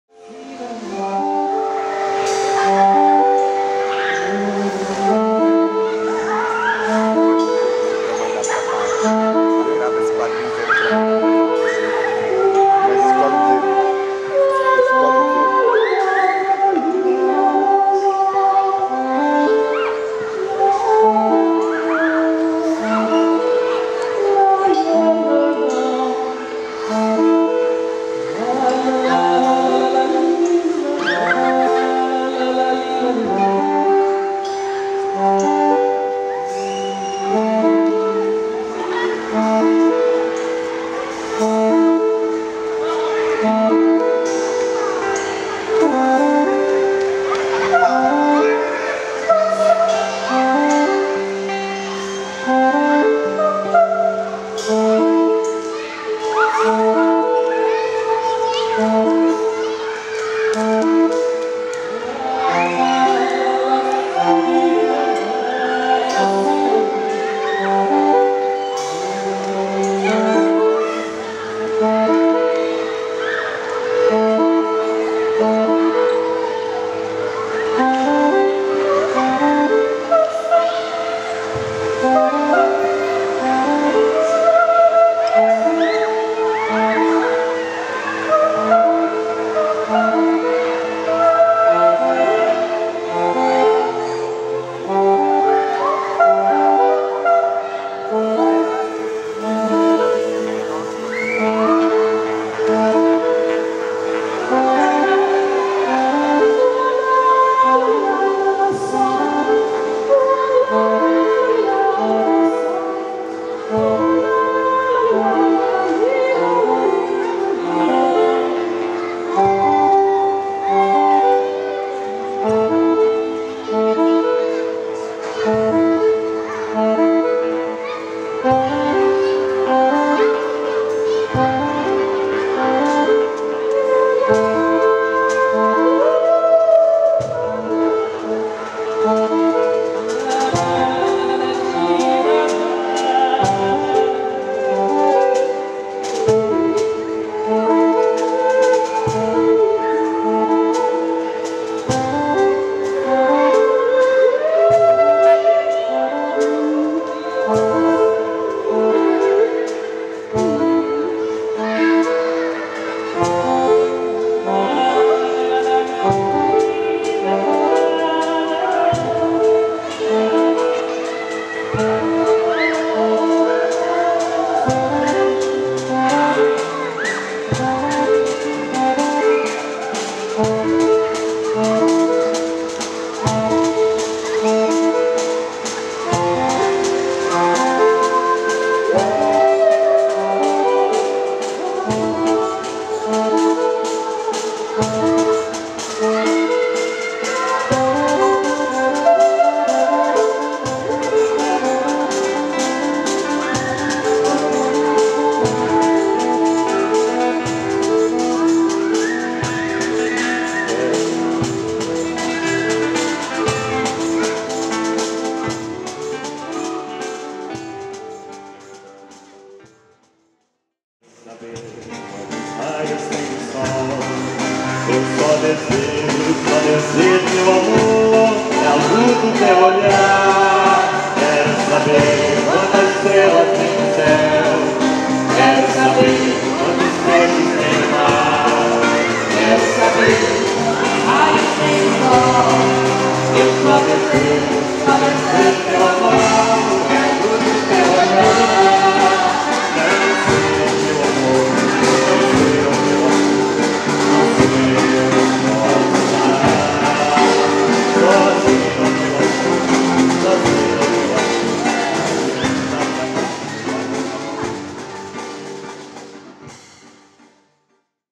Jam_Session_Perversos_Toada_Sesc_2012.mov
Jam session feita no Sesc Pinheiros em fevereiro 2012 pela Cia. Perversos Polimorfos e Cia. do Tijolo - Núcleo Toada